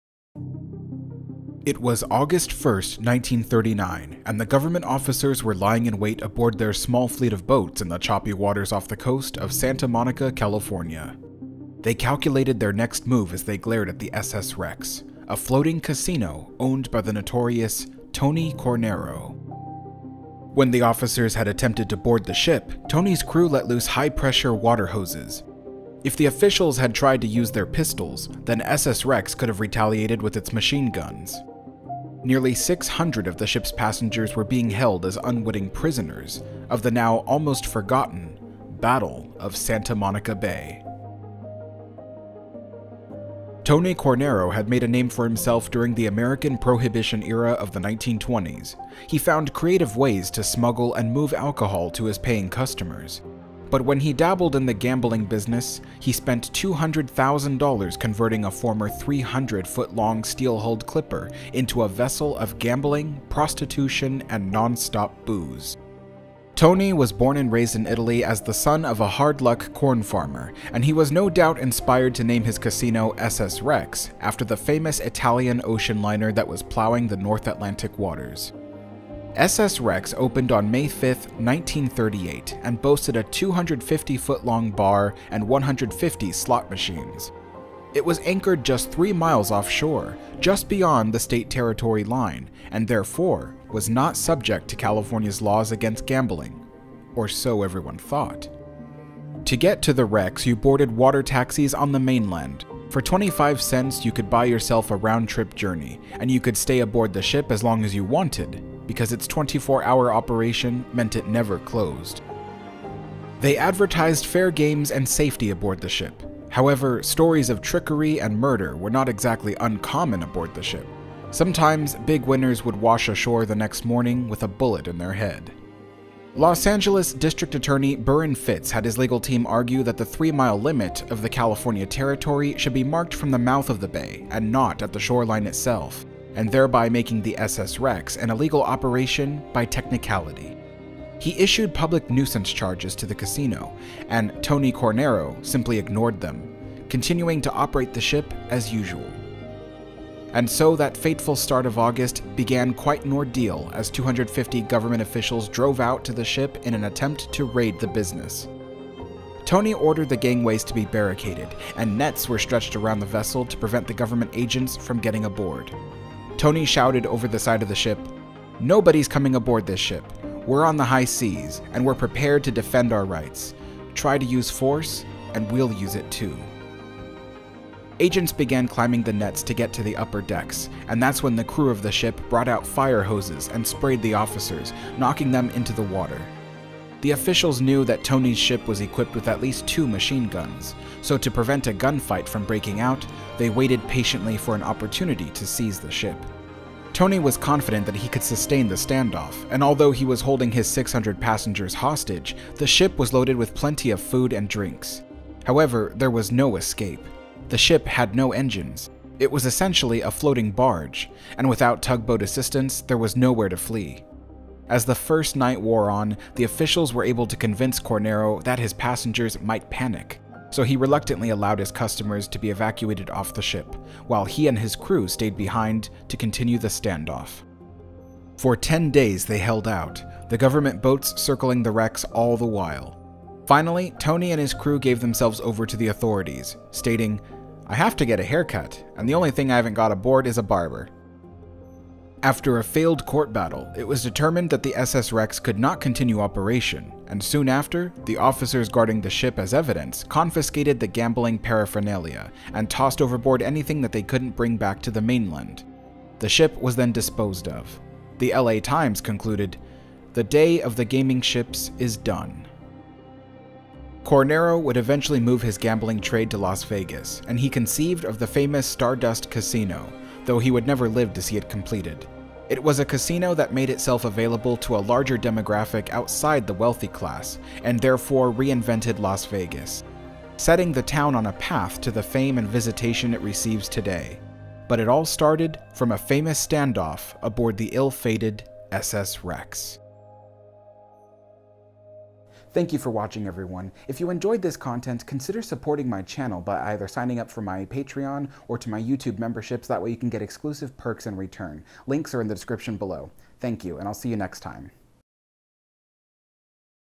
SS Rex and the Battle Of Santa Monica Bay
This is a rather short story but one I thought was an interesting little bit of forgotten history. This is about the SS Rex gambling casino ship that once floated off the waters of Santa Monica, California and the subsequent standoff that ensued when police tried to raid it. This event was really and truly referred to as "The Battle of Santa Monica Bay" so that is not some clickbait title I made up. While the battle was not fought with guns and knives, it was fought with water cannons and patience. Patience, of course, won.

YouTube Membership: Join my exclusive YouTube Membership by clicking "Join" on my YouTube home page.